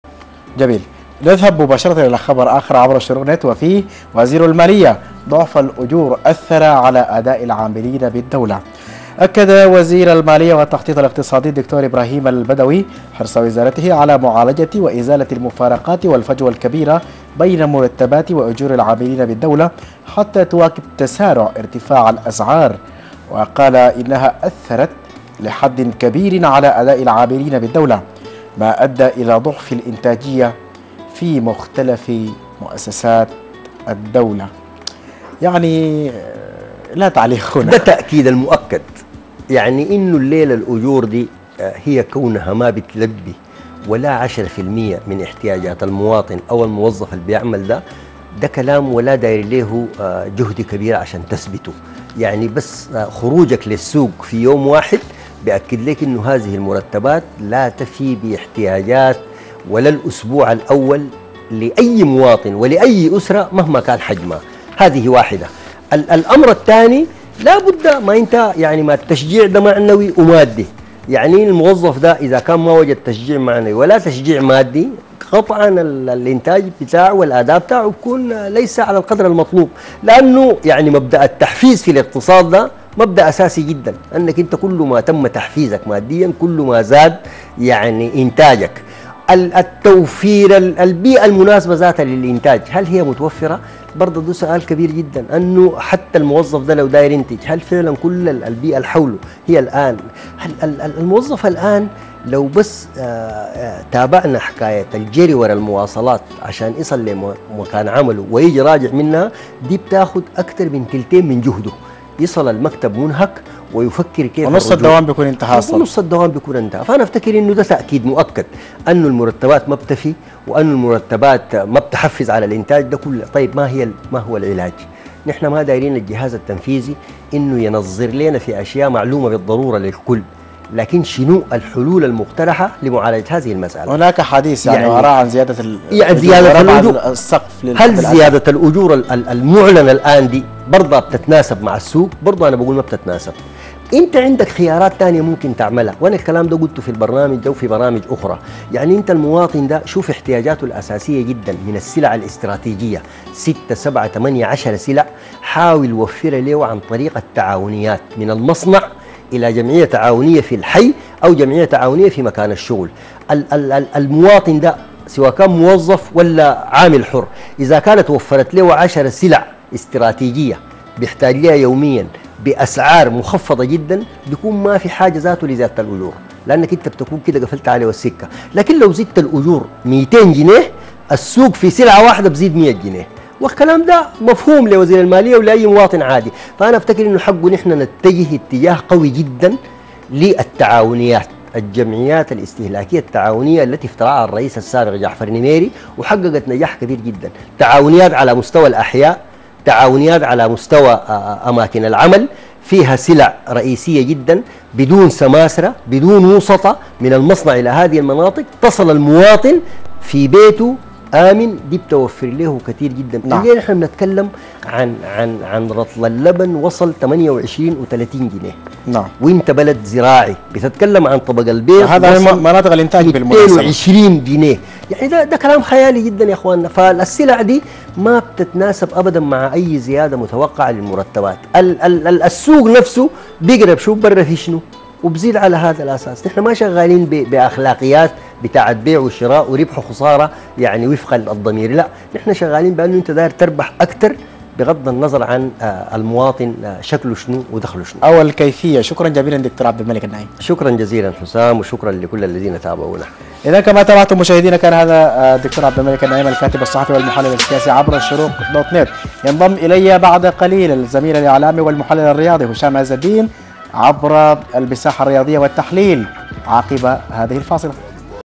وزير المالية : ضعف الأجور أثر على أداء العاملين بالدولة
تحليل الخبر مع د/ عبد الملك النعيم ـ صحفي وكاتب ومحلل سياسي
ـــــــــــــــــــــــــــــــــــــــــــــــــــــــــــــ

الشروق_ الفضائية
Ashorooq Tv
ـــــــــــــــــــــــــــــــــــــــــــــــــــــــــــــ